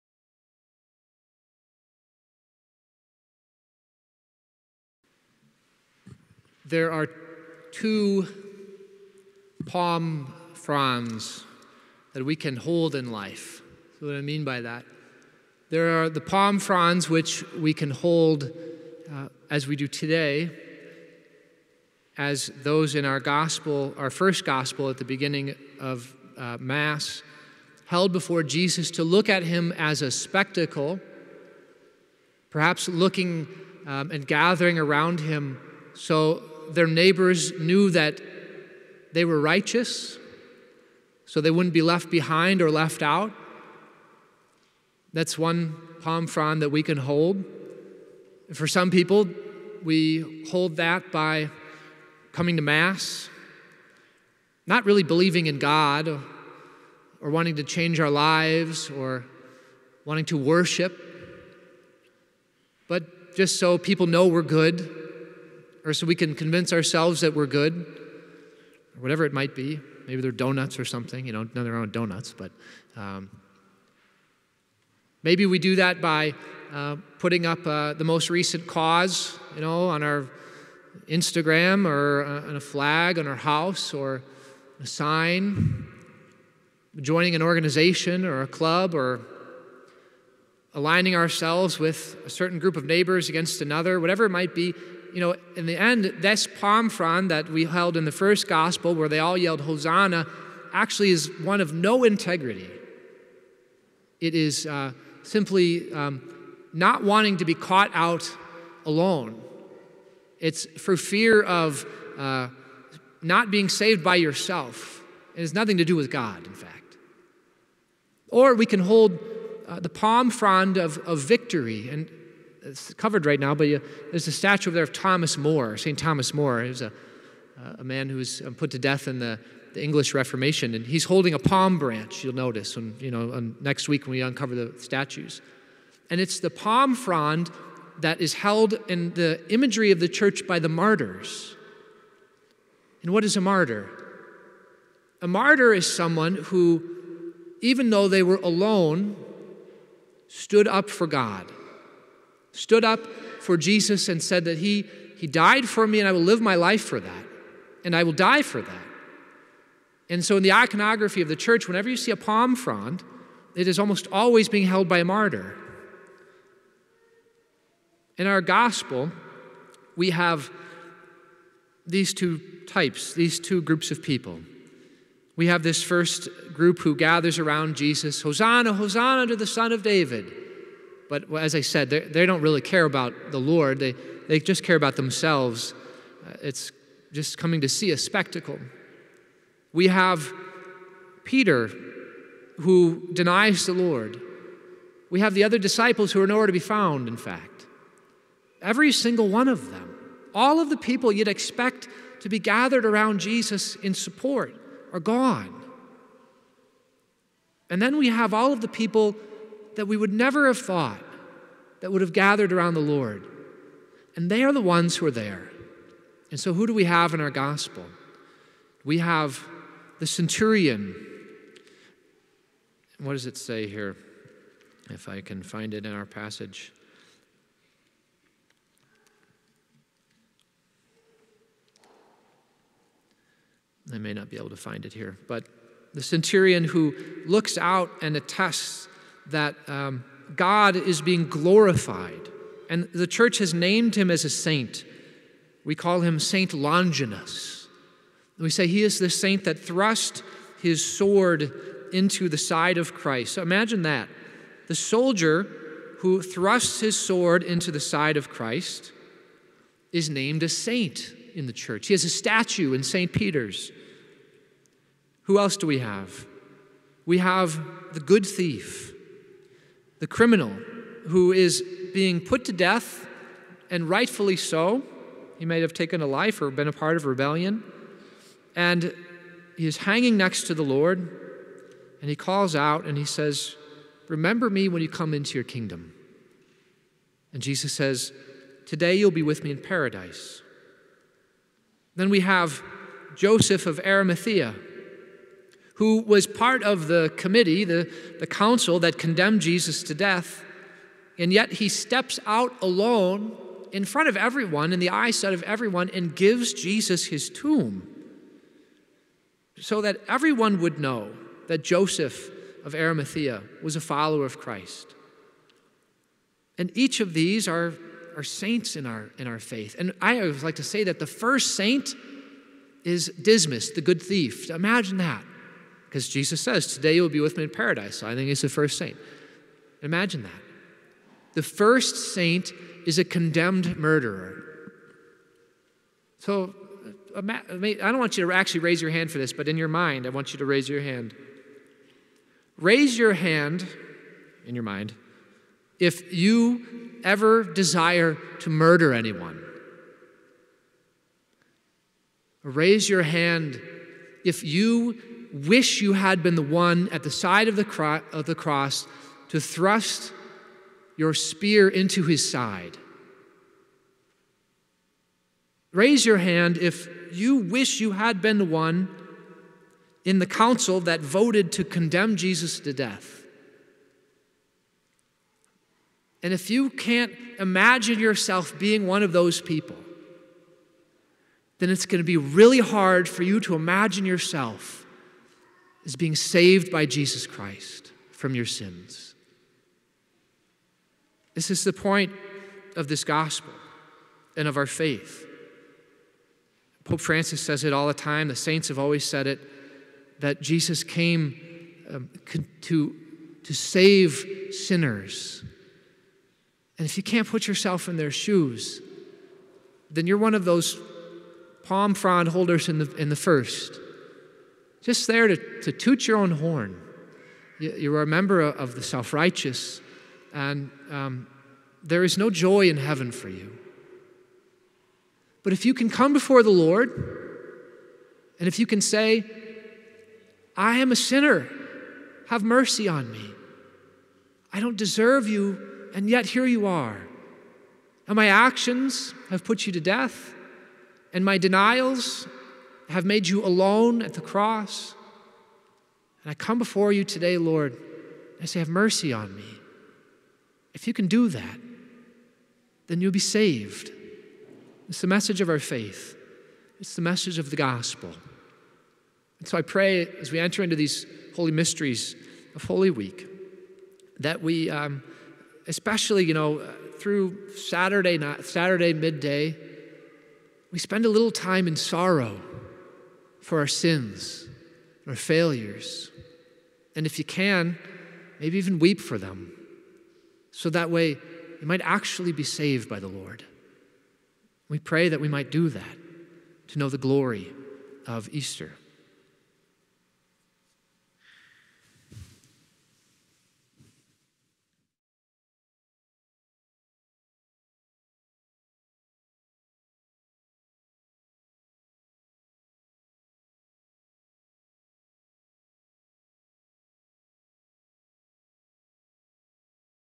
Homily - Palm Sunday, 2022
At St. Robert of Newminster Parish, Shorewood, WI
Presider: Fr. Andrew Infanger

Procession - Gospel: Lk 19:28-40
Reading I: Is 50:4-7
Response: Ps 22:8-9, 17-18, 19-20, 23-24
Reading II: Phil 2:6-11
Gospel: Lk 22:14—23:56